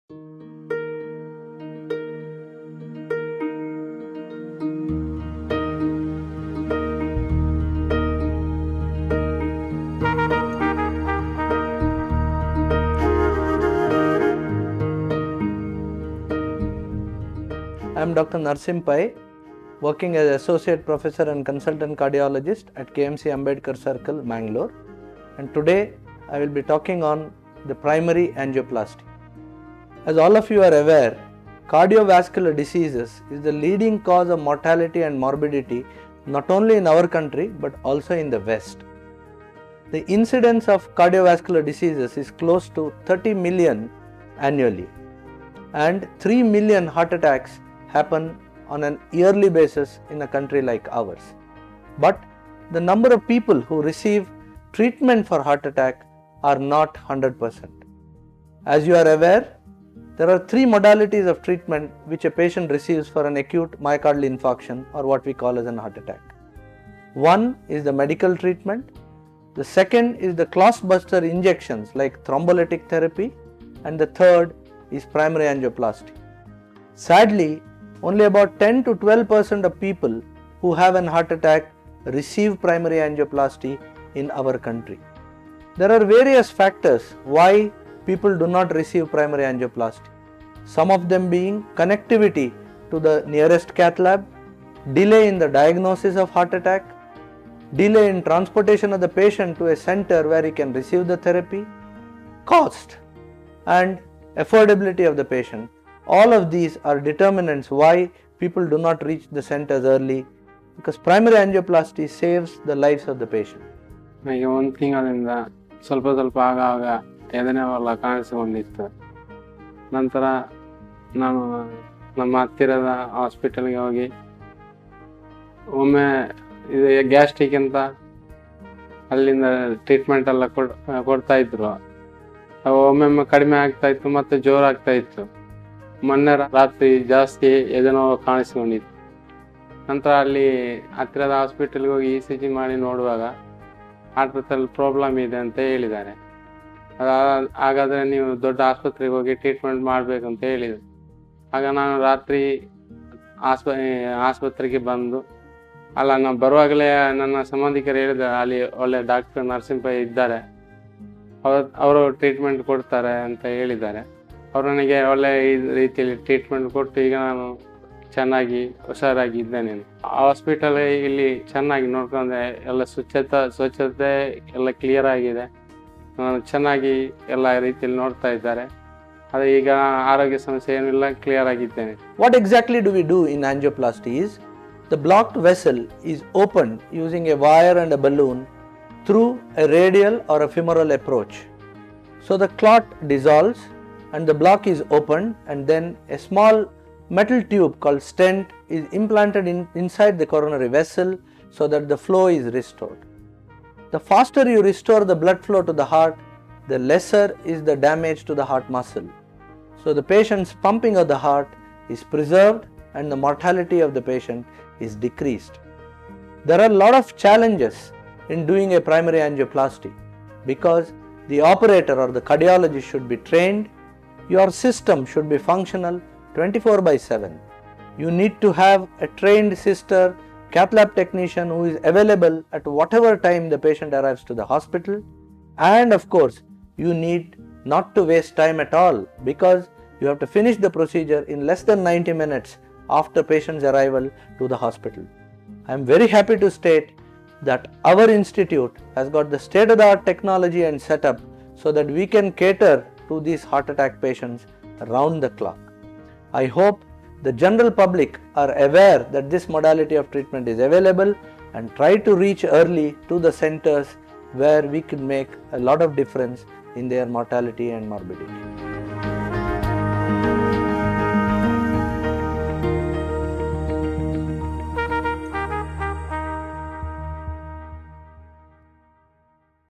Dr. Narasimha Pai on Coronary Angioplasty | CABG Bypass Surgery | Heart Problems Symptoms
Watch Dr. Narasimha Pai, Consultant-Cardiologist, KMC Hospital, Mangaluru, throw light on coronary angioplasty, the process of the treatment & cabg bypass surgery and englighten us to various heart problems &  symptoms, why heart attack symptoms happens, what are the  causes for heart attacks,  and different signs of heart attacks.